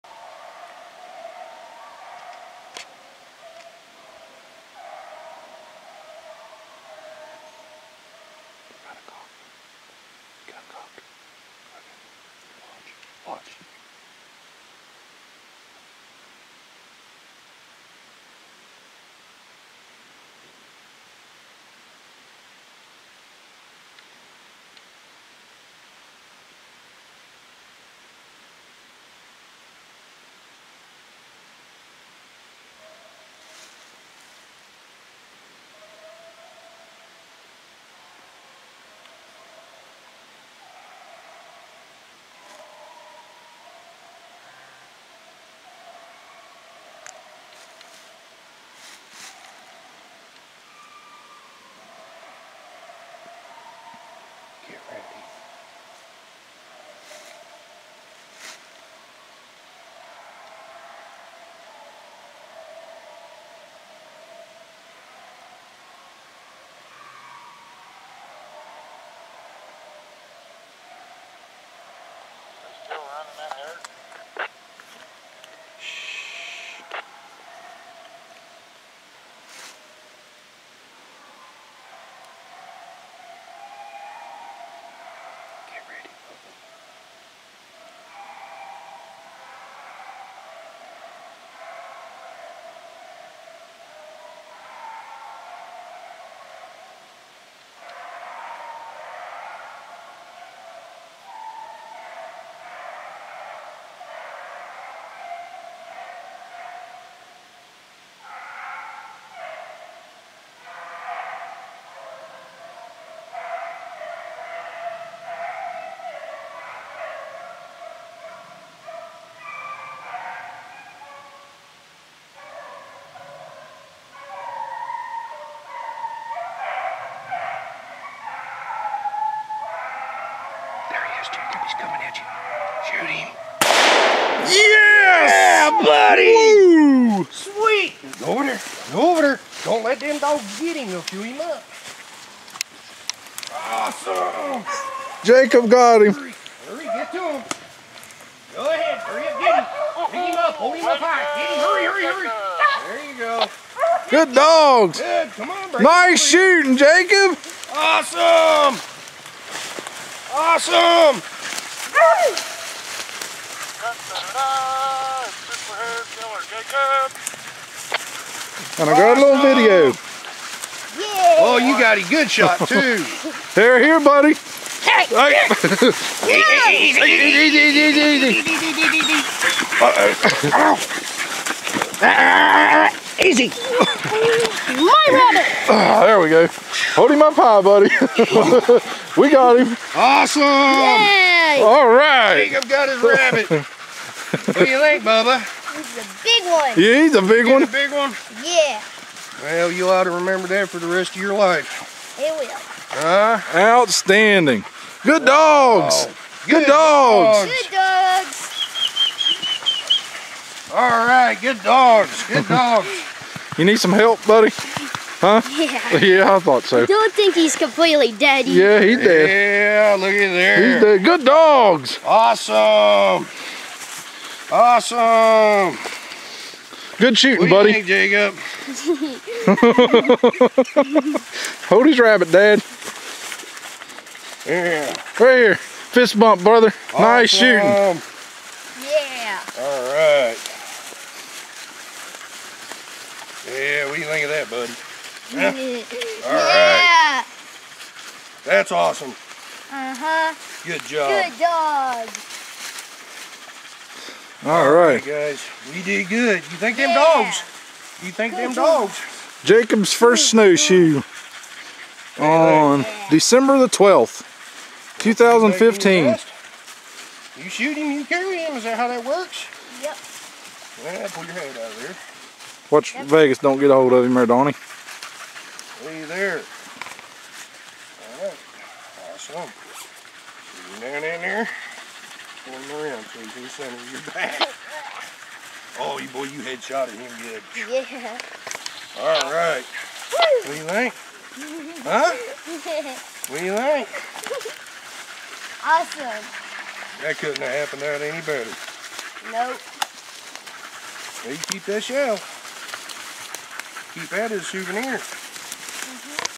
Spruce Run Beagles Hare Hunt 12-12-15
11 year olds first snowshoe hare. Kill Shot!!!